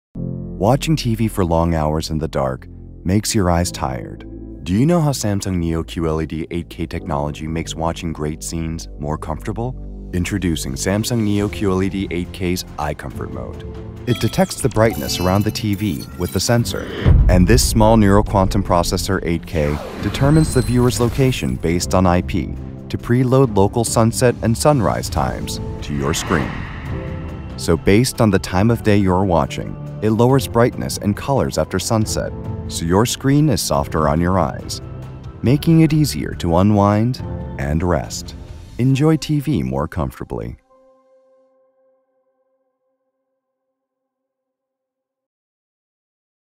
Neo QLED 8K (2022): Einzigartiger Augenkomfort-Modus | Un mode de confort oculaire unique | Samsung
Der Eye Comfort Mode passt das Licht automatisch der Umwelt an und schont so die Augen.

Le mode Eye Comfort adapte automatiquement la lumière à l'environnement et protège ainsi les yeux.